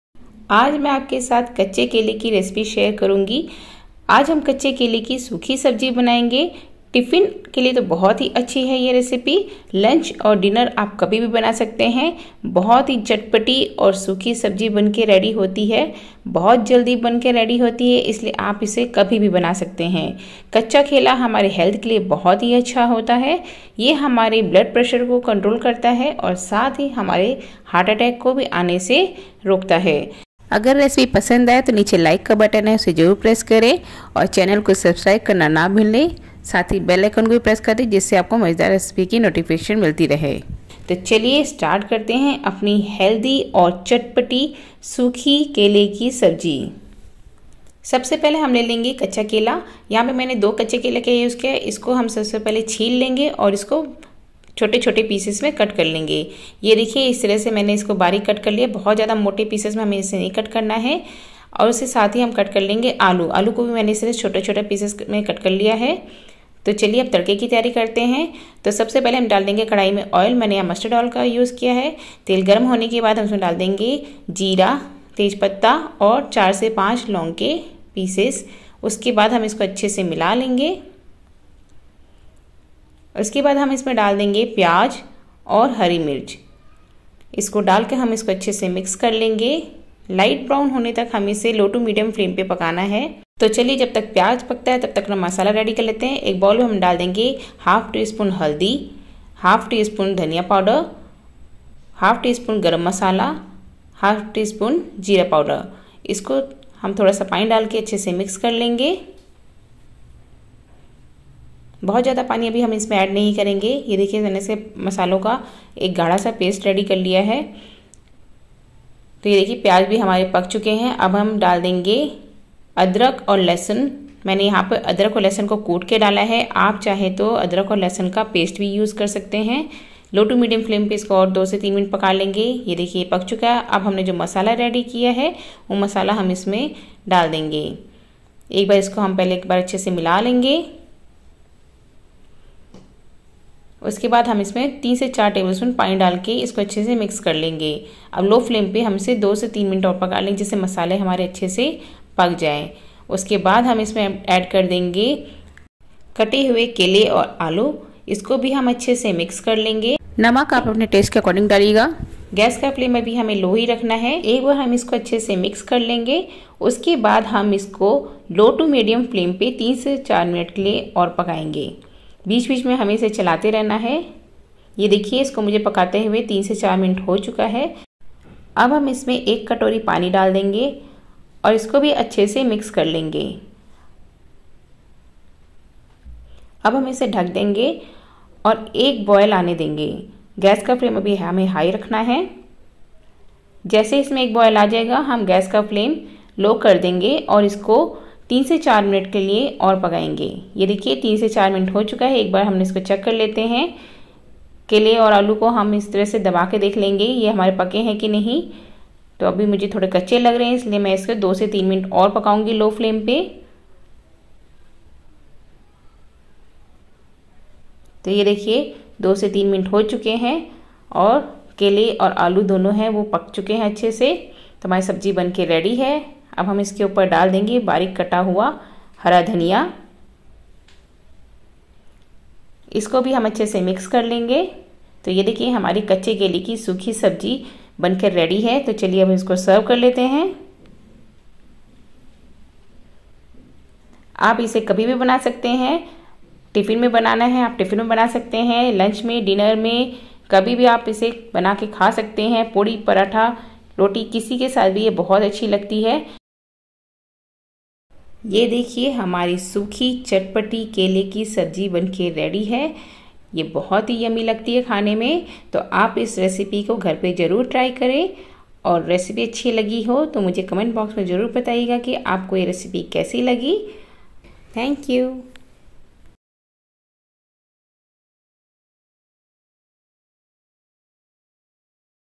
कच्चे केले की मसालेदार सूखी सब्जी ऐसे बनाऐगे तो सभी तारीफ करेगे l Kele Ki Sabji l Raw Banana Recipe
कच्चे केले की मसालेदार सूखी सब्जी ऐसे बनाऐगे तो सभी तारीफ करेगे l Kacche Kele Ki Sabji l Raw Banana Recipe l Kele ki sabji l Kele ki recipe

Ingredients -
Raw banana - 2
Potatoes -2
Jeera - 1 tsp
Bay leaf - 1-2 pieces
Cloves- 4-5 pieces
Onion - 1 medium size
Chilli as per taste
Ginger - 1 inch
Garlic - 4-5 pieces
Coriander

Aaj mai aapse kacche kele ki sukhi chatpati sabji ki recipe share kr rahi hu. Aap iski tiffin k liye bna sakte hai. Bhut jaldi ban kr ready hoti hai. Aap kacche kele ki sabji ko ek bar es terah se banaye. Recipe pasand aaye to 👍 kre or share krna na bhule..